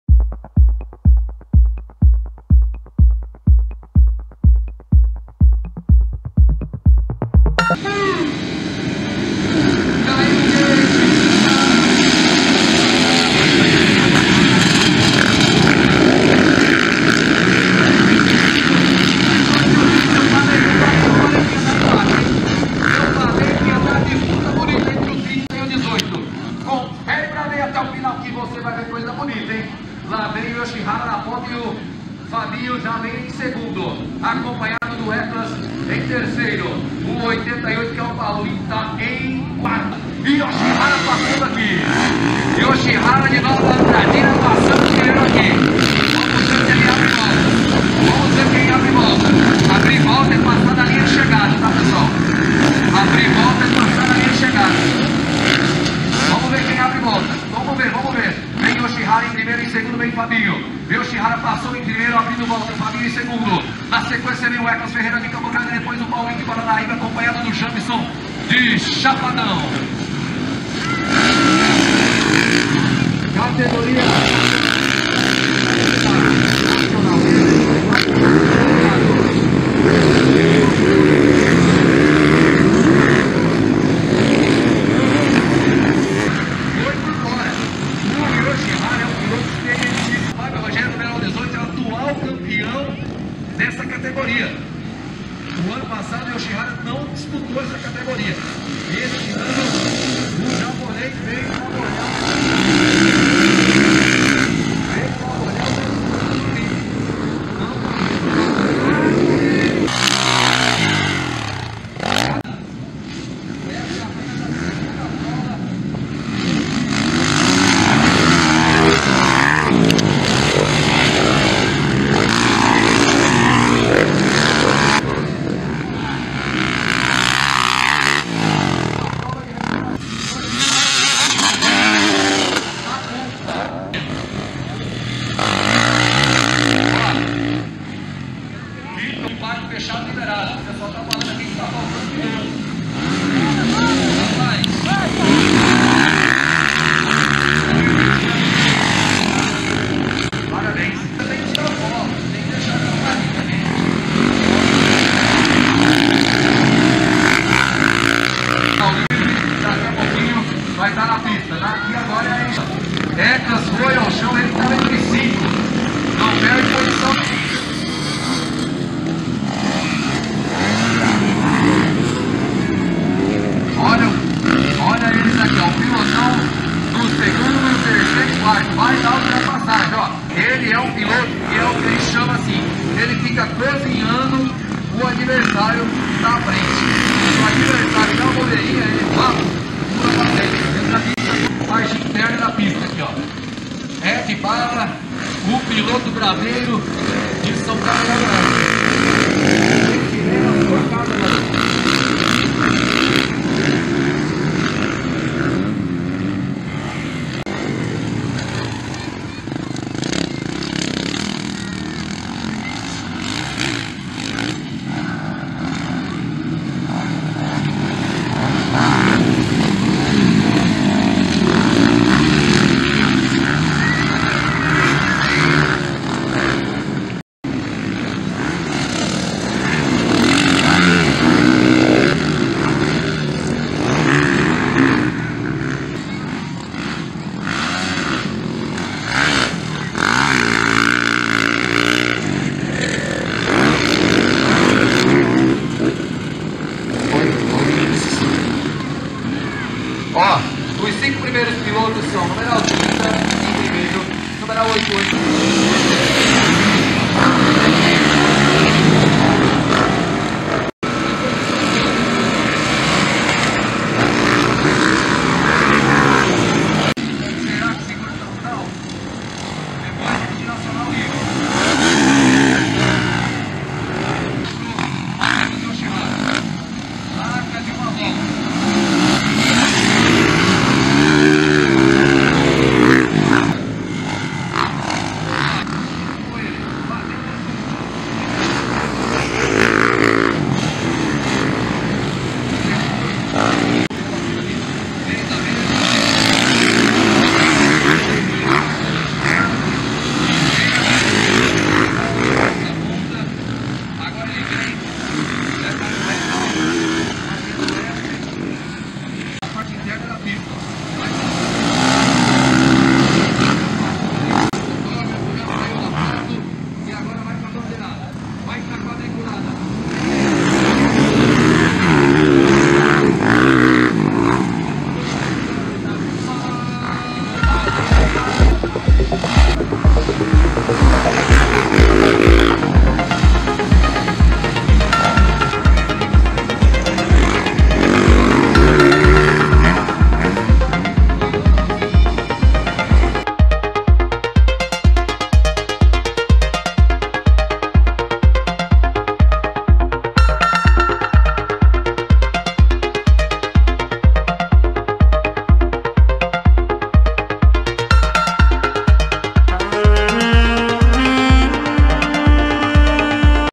MX5 em Rochedo pela 3º etapa do Campeonato Estadual Sul-mato-grossense de Motocross 2024
Etapa disputada em Rochedo (MS) em 14 de abril de 2024

Essa produção ocorreu graças aos parceiros do blog @TVHoleshot